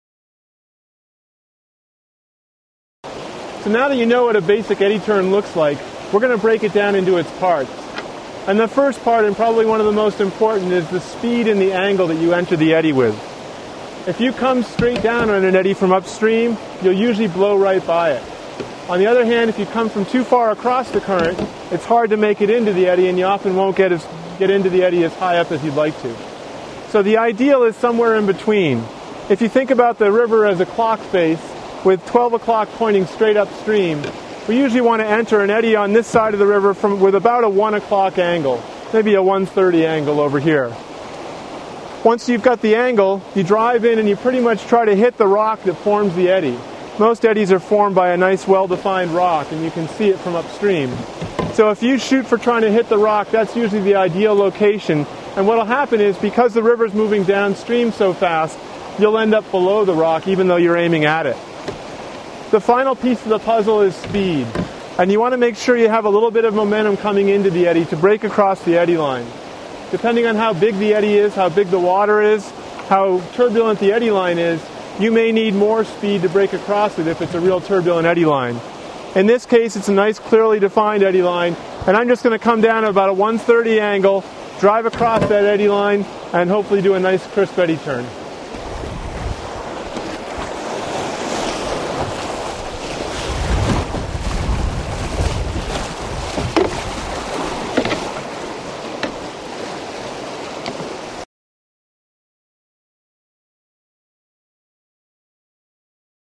Speed & Angle for Eddy Turns in a White Water Canoe
Speed & Angle for Eddy Turns in a White Water Canoe. Part of the series: Basic Maneuvers for Solo White Water Canoeing. Speed and angle are two of the most important aspects of how to do an eddy turn in a solo white water canoe. Get tips on how to stop a canoe in rapids with an eddy turn in this free online white water canoeing maneuvers lesson on video.